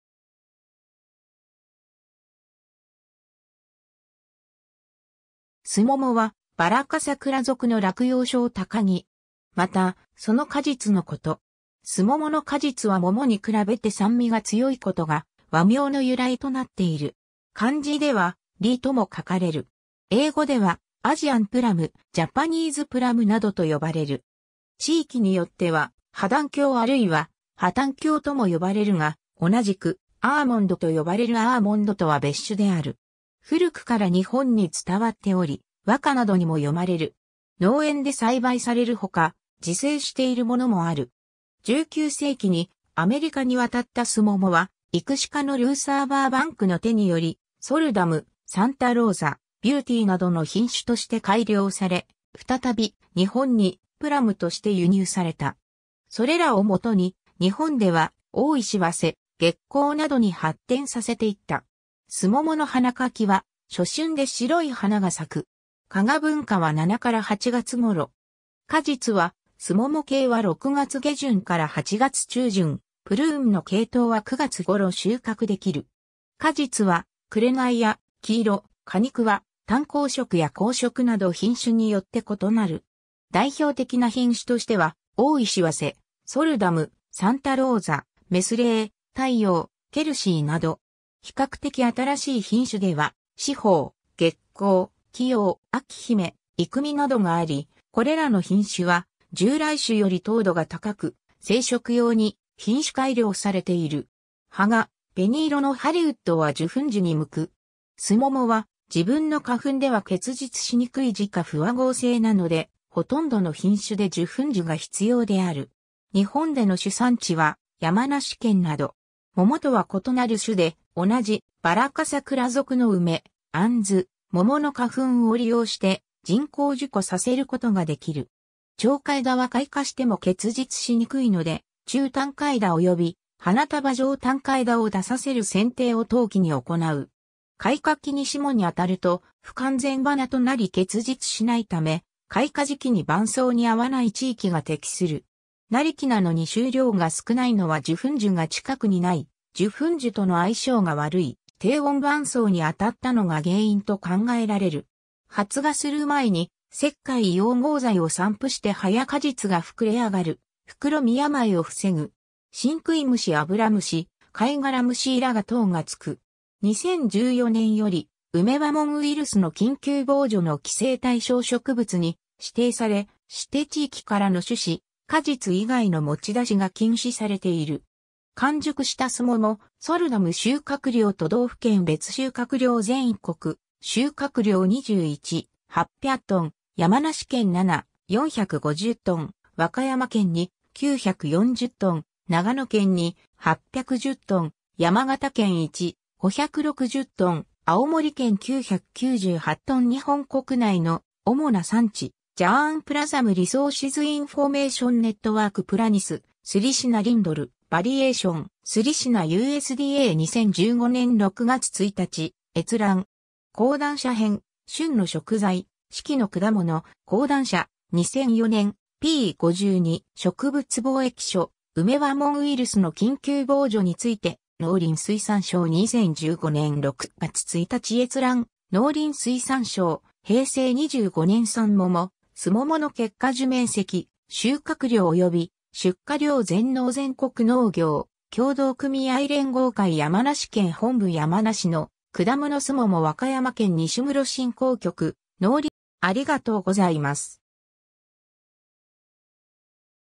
スモモ
スモモ（李、酢桃、学名：Prunus salicina）はバラ科サクラ属の落葉小高木。
また、その果実のこと。
 スモモの果実はモモに比べて酸味が強いことが、和名の由来となっている。
漢字では「李」とも書かれる。
英語では「Asian plum」「Japanese plum」などと呼ばれる（ただしウメも「プラム」と呼ばれることがある）。
地域によっては、ハダンキョウあるいはハタンキョウ（巴旦杏）とも呼ばれるが、同じく巴旦杏と呼ばれるアーモンドとは別種である。
古くから日本に伝わっており、和歌などにも詠まれる。
農園で栽培される他、自生しているものもある。
 19世紀にアメリカに渡ったスモモは育種家のルーサー・バーバンクの手により「ソルダム」、「サンタローザ」、「ビューティー」などの品種として改良され、再び日本に「プラム」として輸入された。
それらを元に日本では「大石早生」、「月光」などに発展させていった。
 スモモの花 花期は初春で白い花が咲く。
花芽分化は7 - 8月頃。
果実はスモモ系は6月下旬から8月中旬、プルーンの系統は9月頃収穫できる。
果実は紅や黄色、果肉は淡黄色や紅色など品種によって異なる。
代表的な品種としては「大石早生」、「ソルダム」、「サンタローザ」、「メスレー」、「太陽」、「ケルシー」など。
比較的新しい品種では「紫峰」、「月光」、「貴陽」、「秋姫」、「いくみ」などがあり、これらの品種は従来種より糖度が高く、生食用に品種改良されている。
葉が紅色のハリウッドは受粉樹に向く。
スモモは自分の花粉では結実しにくい自家不和合性なのでほとんどの品種で受粉樹が必要である。
日本での主産地は山梨県など。
 桃とは異なる種で、同じバラ科サクラ属の梅、杏、桃の花粉を利用して人工授粉させることができる。
長果枝は開花しても結実しにくいので、中短果枝および花束状短果枝を出させる剪定を冬季に行う。
開花期に霜に当たると、不完全花となり結実しないため、開花時期に晩霜に遭わない地域が適する。
成木なのに収量が少ないのは受粉樹が近くにない・受粉樹との相性が悪い・低温晩霜に当たったのが原因と考えられる。
発芽する前に石灰硫黄合剤を散布して葉や果実が膨れ上がるふくろみ病を防ぐ。
シンクイムシ・アブラムシ・カイガラムシ・イラガ等がつく。
 2014年（平成22年）より、ウメ輪紋ウイルス（プラムポックスウイルス）の緊急防除の規制対象植物に指定され、指定地域からの種子、果実以外の持ち出しが禁止されている。
 完熟したスモモ「ソルダム」 収穫量 都道府県別収穫量（2013年） 全国収穫量 21,800t 山梨県 7,450t (34.17%) 和歌山県 2,940t (13.48%) 長野県 2,810t (12.89%) 山形県 1,560t (7.16%) 青森県 998t (4.58%) 日本国内の主な産地（作況調査市町村別データ長期累年一覧による） ^ Germplasm Resources Information Network "Prunus salicina Lindl. var. salicina" USDA 2015年6月1日閲覧。
 ^ 講談社編『旬の食材：四季の果物』、講談社、2004年、p.52. ^ 植物防疫所「ウメ輪紋ウイルス（プラムポックスウイルス）の緊急防除について」 農林水産省 2015年6月1日閲覧 ^ 農林水産省 平成25年産もも、すももの結果樹面積、収穫量及び出荷量 ^ 全農 全国農業協同組合連合会 山梨県本部 山梨のくだもの すもも ^ 和歌山県 西牟婁振興局農林水産振興部農業水産振興課 すもも